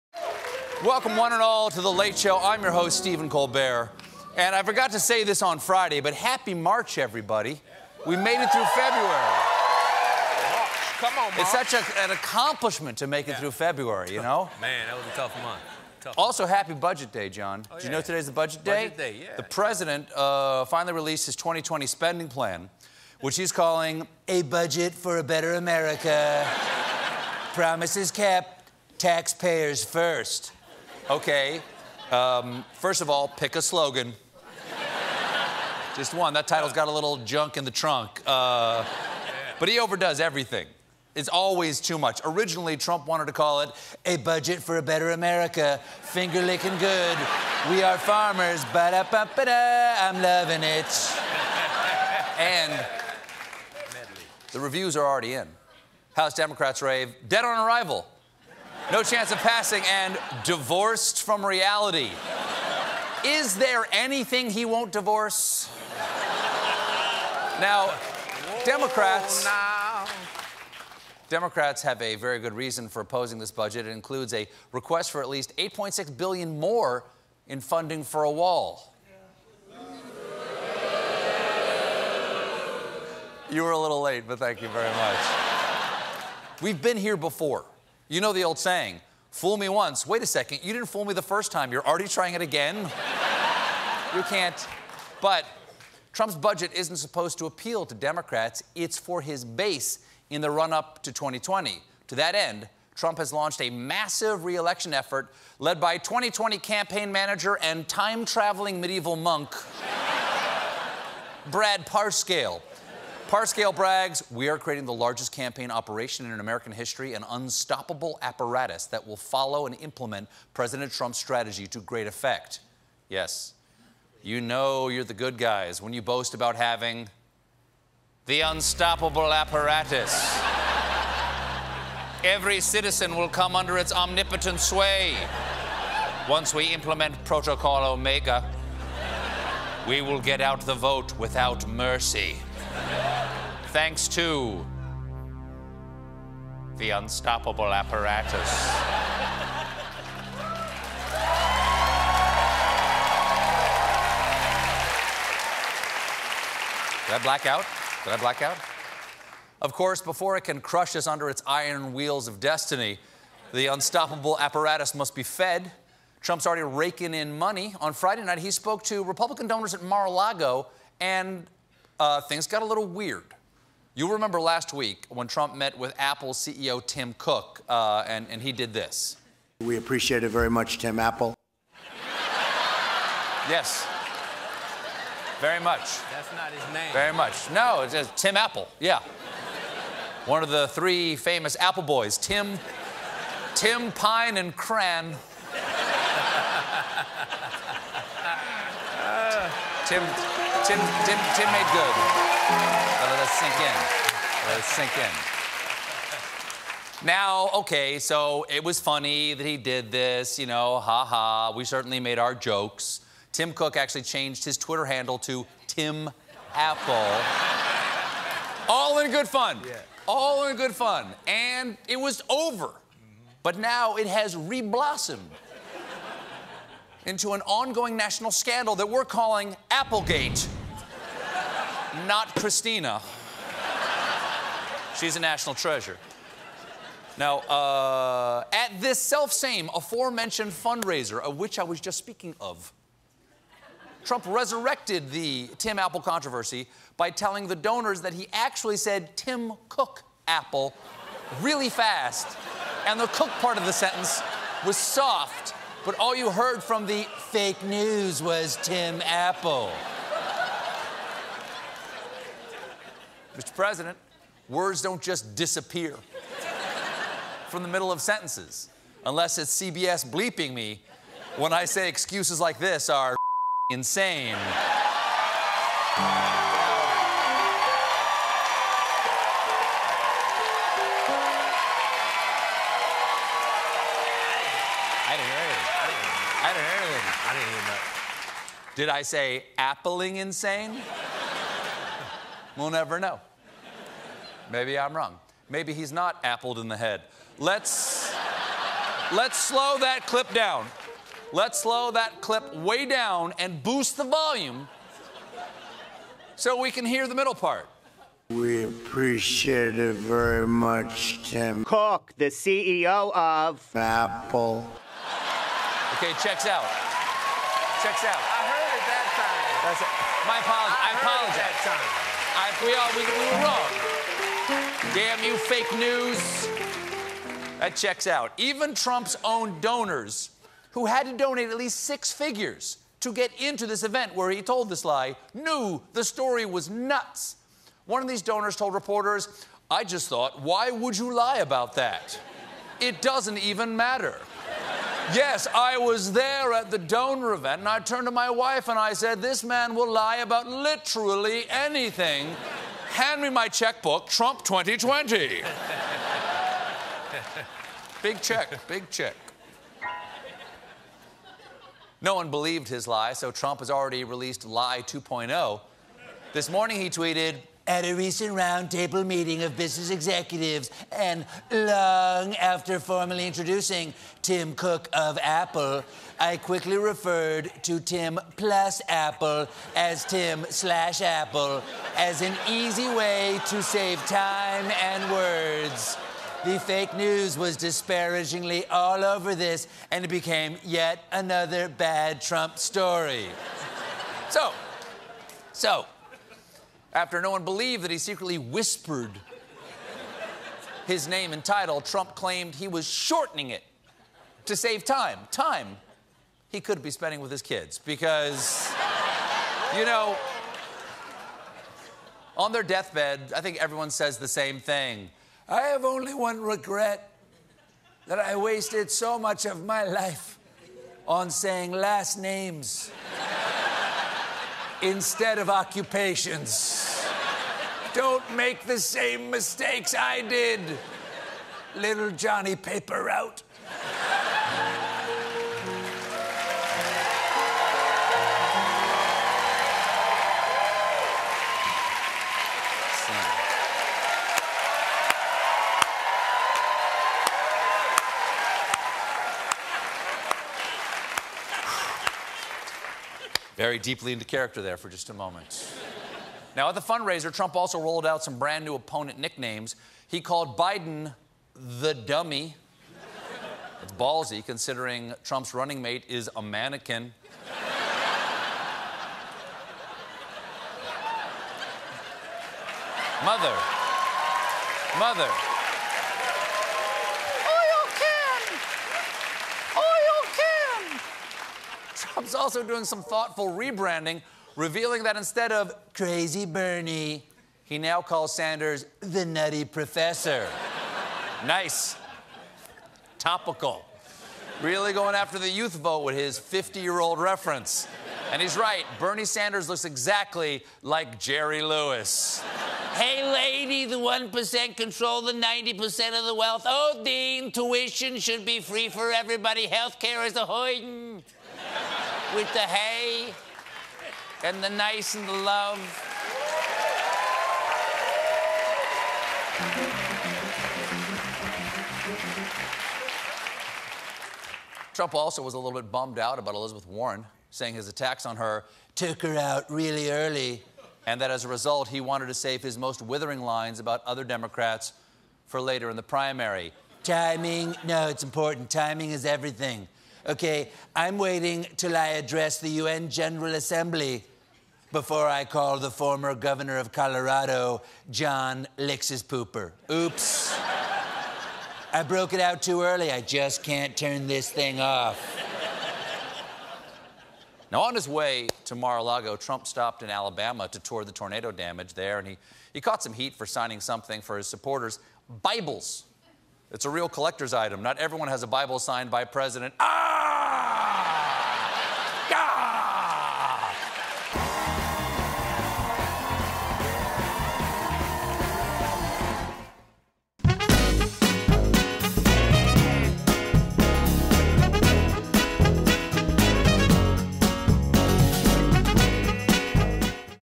New Trump Nicknames For 2020 Democrats
'Crazy Bernie' is retired. The President has a new and equally dumb nickname for the senator from Vermont.